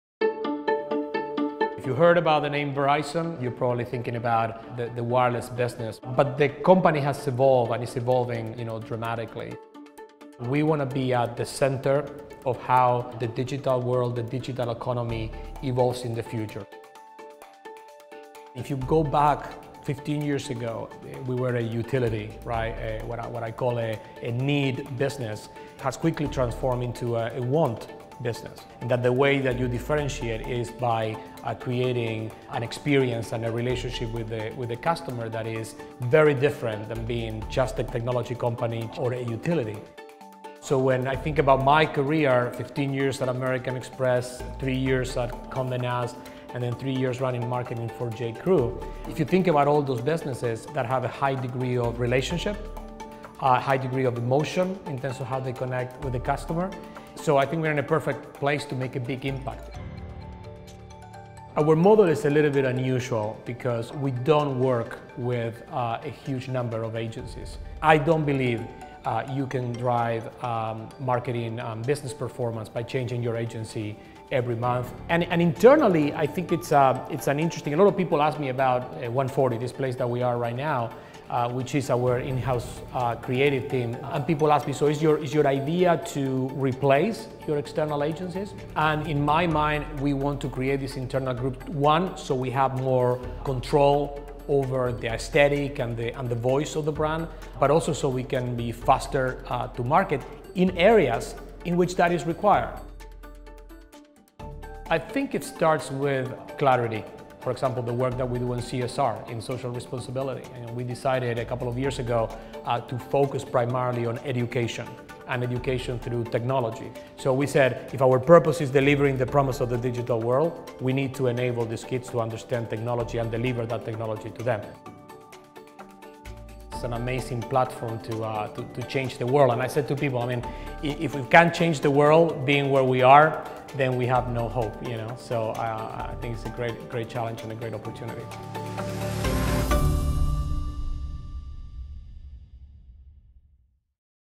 Verizon’s Diego Scotti: Leading Brand and Business Change | Forbes
The communication-technology company’s CMO talks about the new era of Verizon and the purpose and customer relationships he’s cultivating to drive it forward.